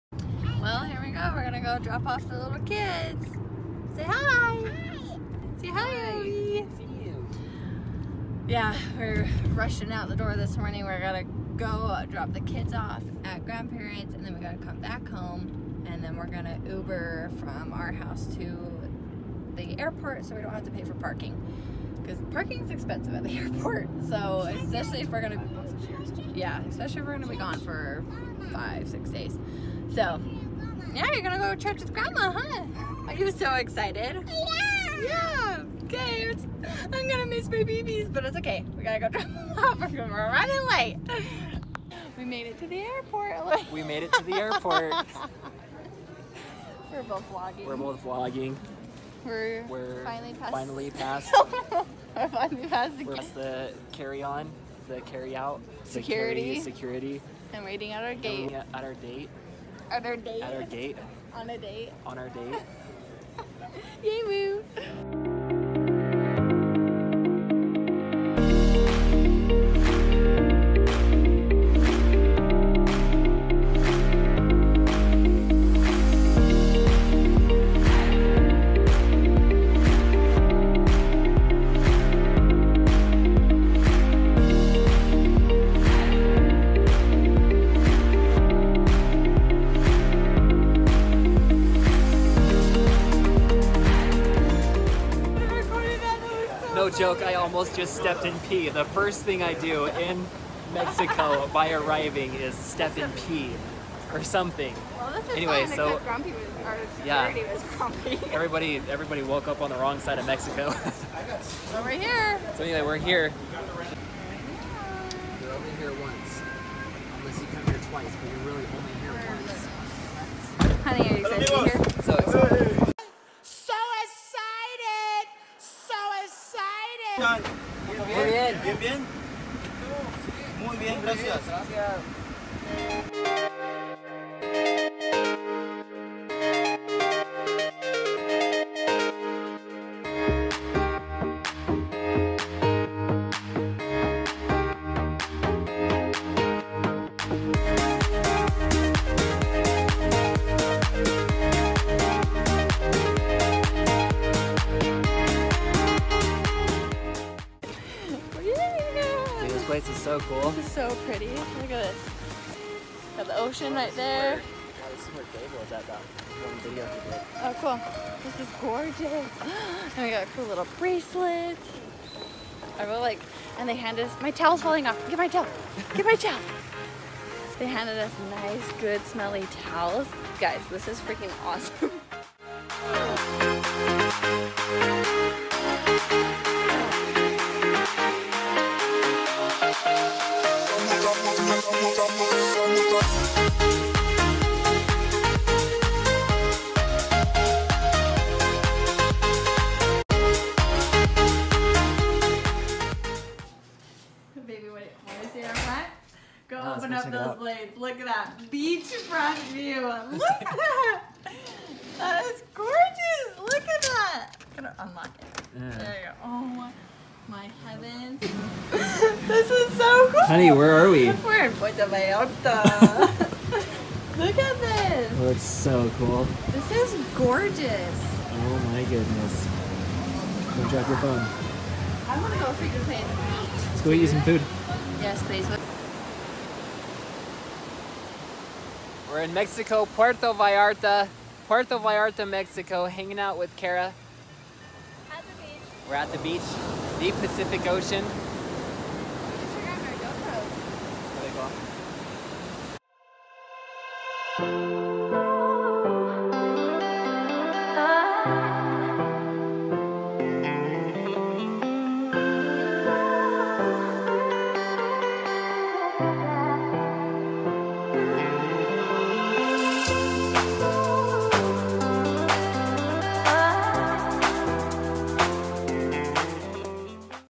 OUR TRIP TO AN ALL-INCLUSIVE RESORT IN PUERTO VALLARTA, MEXICO...With No Kids!
OUR TRIP TO MEXICO...With No Kids!

Hey guys!

This was our first trip without our kids! We missed them so much but we had a wonderful time in Puerto Vallarta! We will defiantly go back soon! Also if you are planning to go to Puerto Vallarta anytime soon we defiantly recommend staying at Marival Armony!